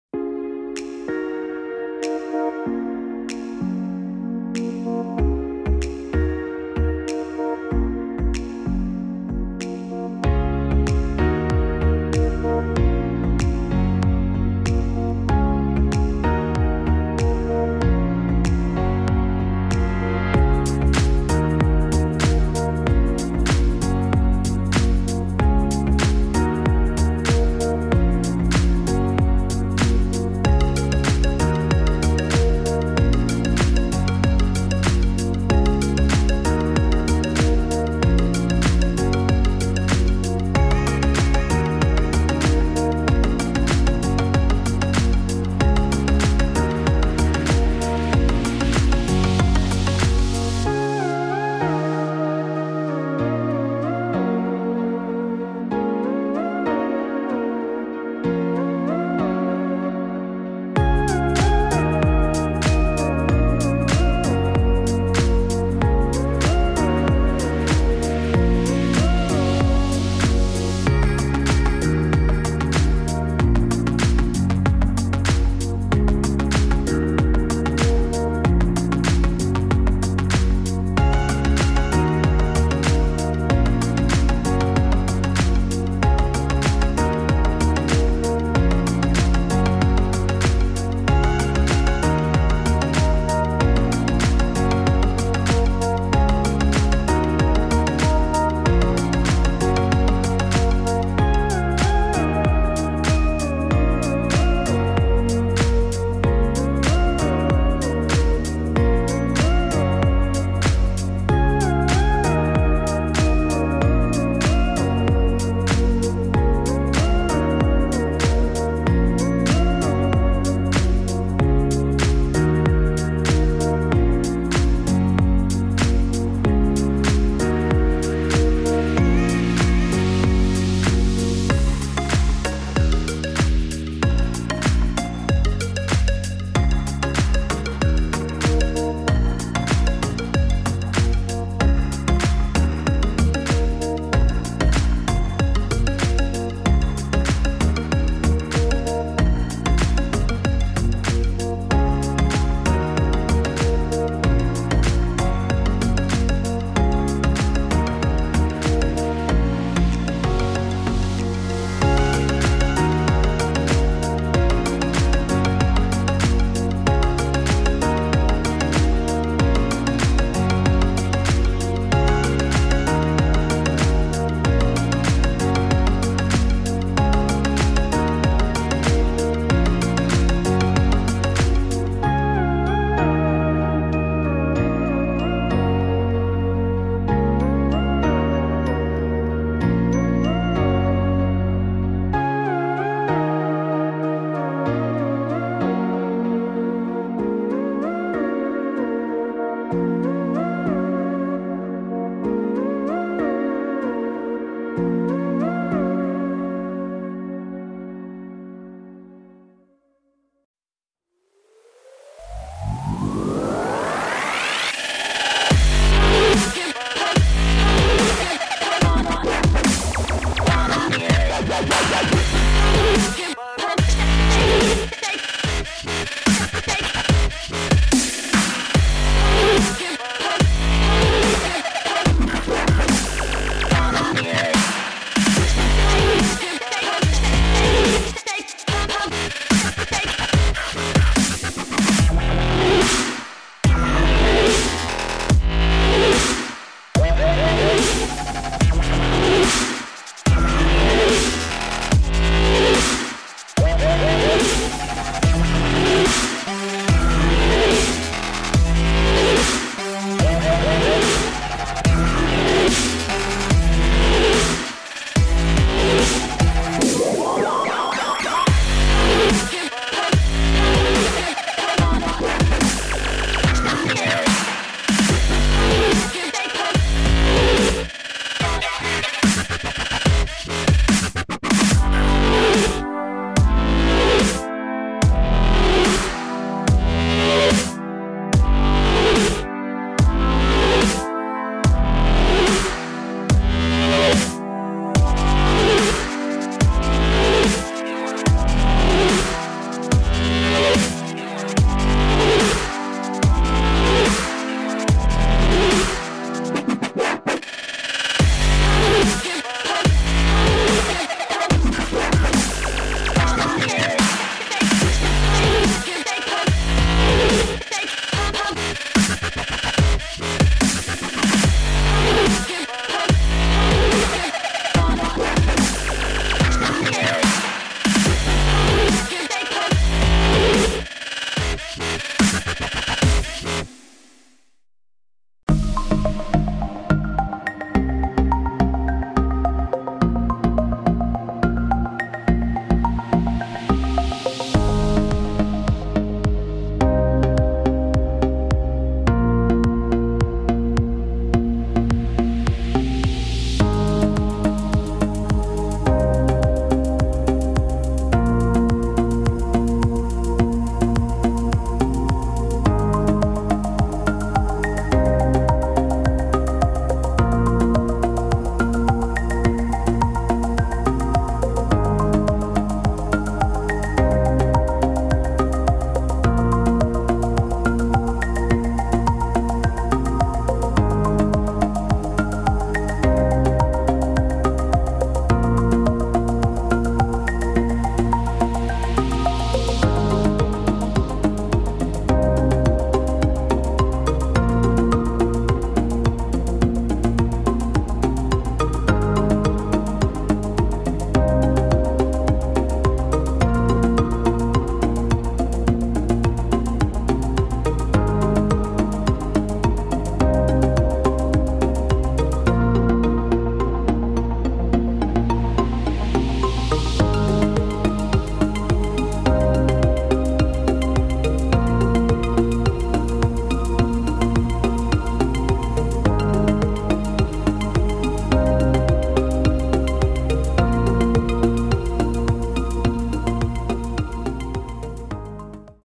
Low poly tank model Blender 2.8 beginner modeling tutorial
Beginner low poly tank modeling tutorial in Blender 2.8

Pc specs:
-cpu: i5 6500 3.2 Ghz
-gpu: nvidia 1060 3Gb
-mobo: asus maximus viii ranger
-ram: 8 Gb HyperX Savage
-memory: 120 Gb ssd + 1 Tb hdd